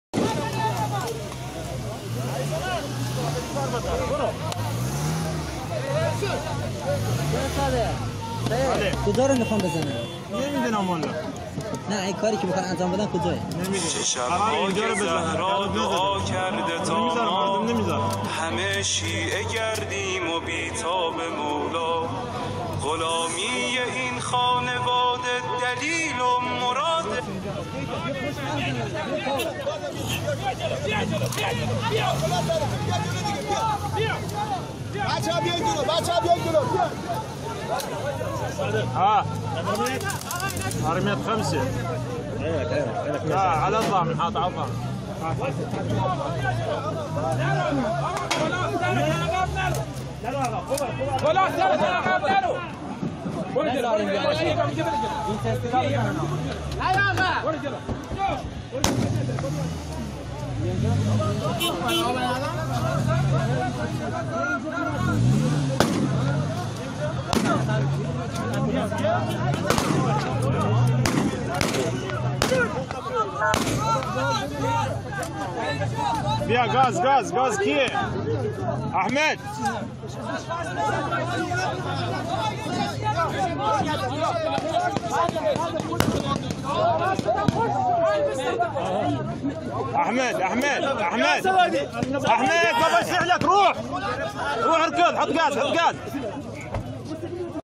سقط قتيل و4 جرحى في إطلاق نار من عناصر الحرس الثوري-3 أبريل2019
سقط قتيل و4 جرحى في إطلاق نار من عناصر الحرس الثوري، الثلاثاء، على مواطنين عرب  الأهواز احتجوا على قيام تلك القوات بإغلاق مسير مياه الفيضانات من أجل عدم وصولها للمنشآت النفطية بينما تهدد الفيضانات القرى بأكملها.